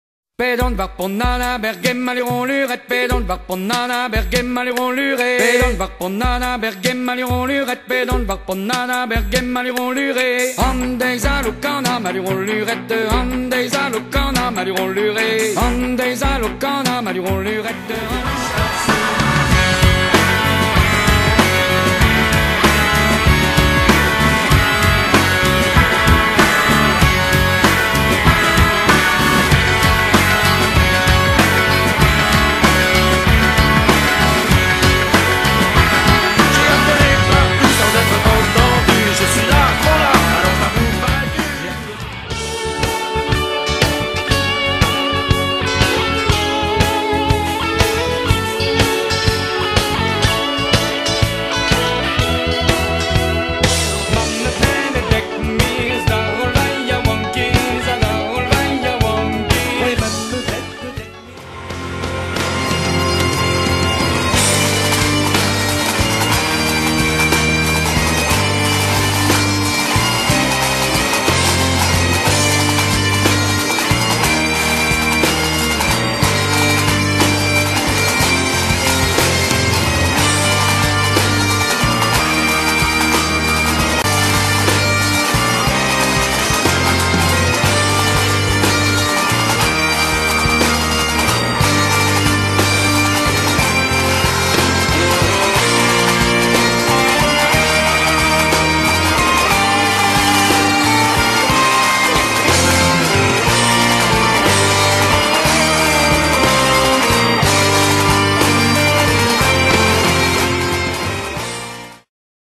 Sonerien Du à Guénin
L'école Notre Dame de Guénin organise son traditionnel Fest-noz le samedi 12 janvier 2013 à la salle multifonctions. Au programme les Sonerien Du, Skolvan et Ehan. Salle chauffée, grillades...